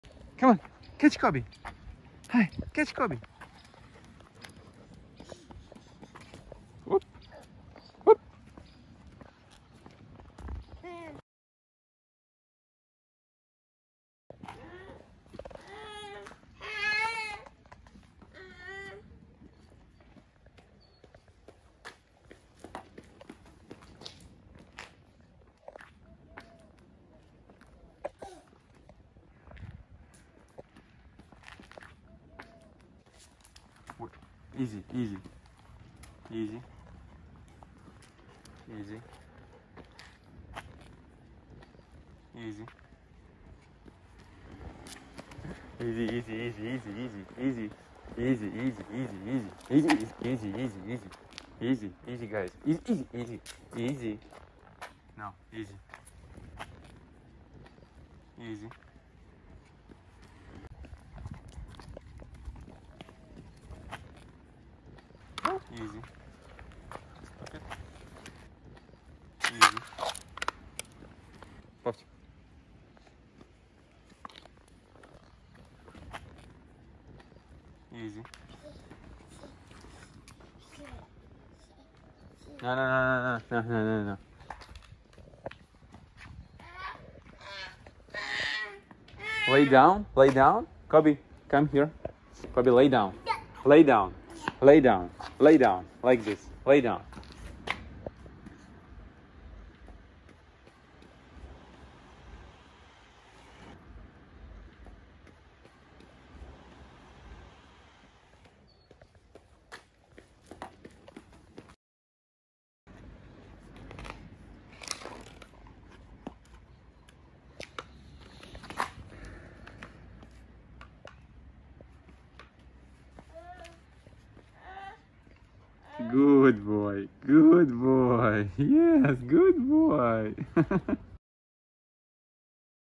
Adorable Moment! Baby Tries to Wake Up Dog for Walk.
Welcome to the Happy Chocolate Family! In today's video, Robert was all set to take Kobe for a walk on a leash. It seemed like a simple task at first, but Kobe had other ideas—namely, taking a little rest! What else can a Labrador do, right? Thanks for joining us!